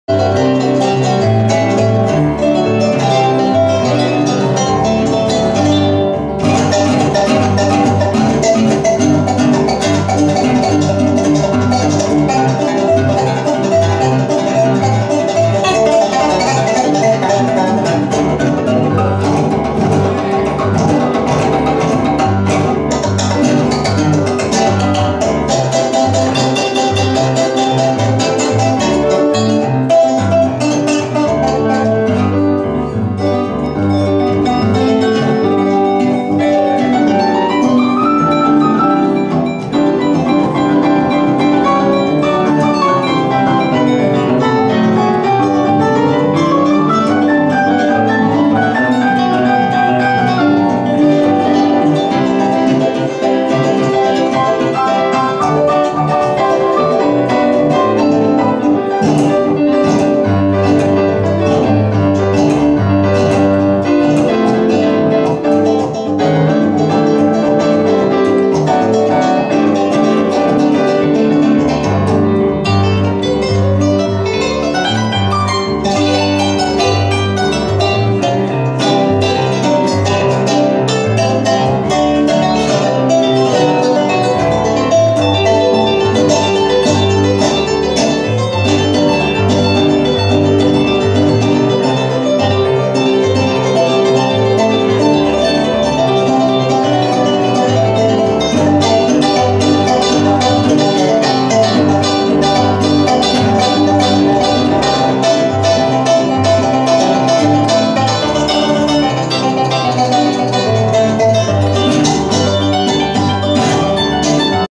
Dúo, Betancourt - Luna, Fragmento de Periquera, Arpa y Piano Bs As
Este es un fragmento de Periquera, ritmo estandar del folklore Venezolano interpretado por Gabriel Luna y Eduardo Betancourt en la peña " La Capilla" en Buenos Aires Argentina 2009 disfrutenla!